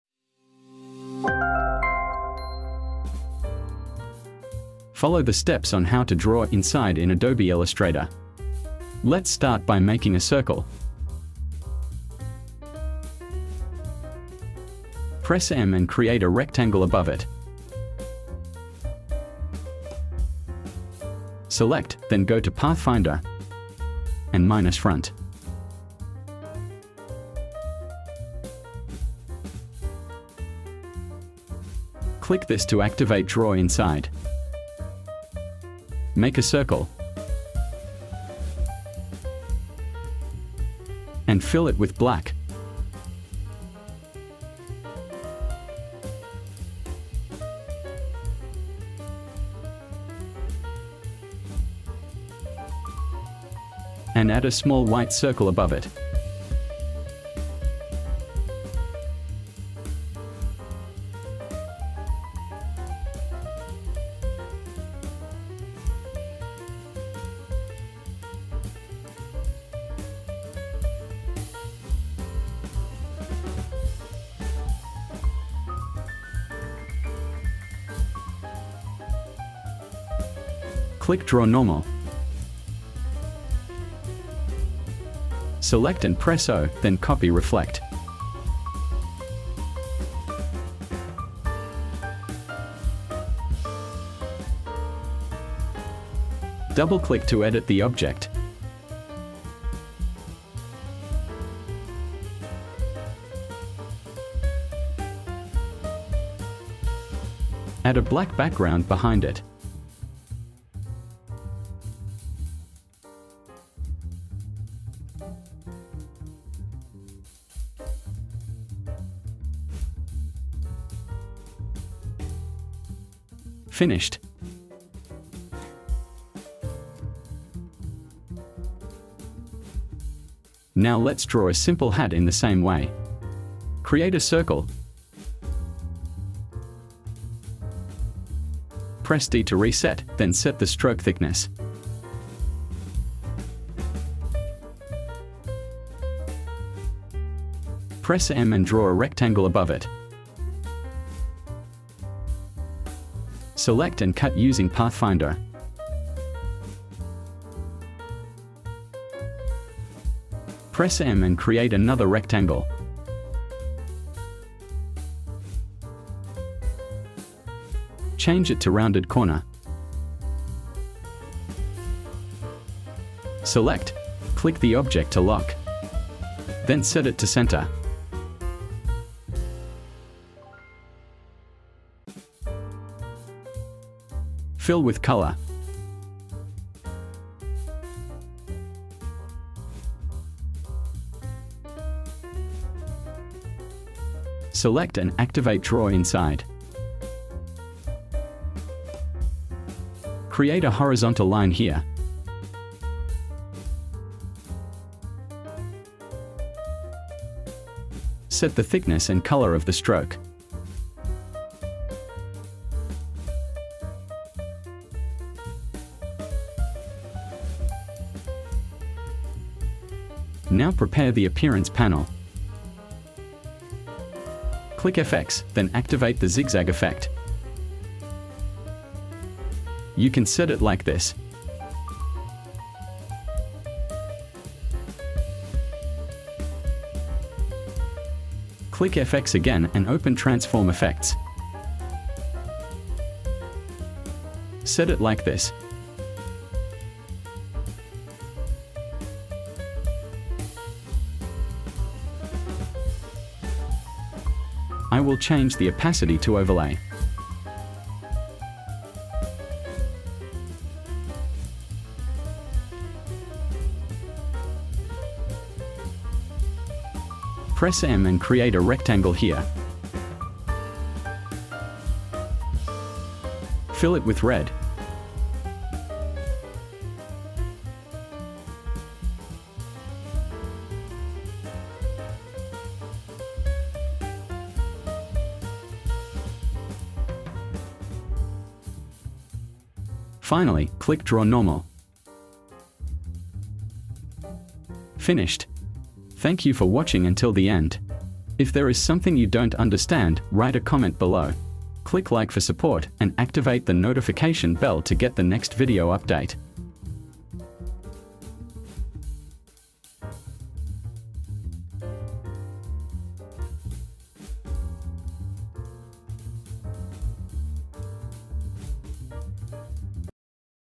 How to Draw inside an Object in Illustrator
this video explains How to Draw Inside an Object in Illustrator.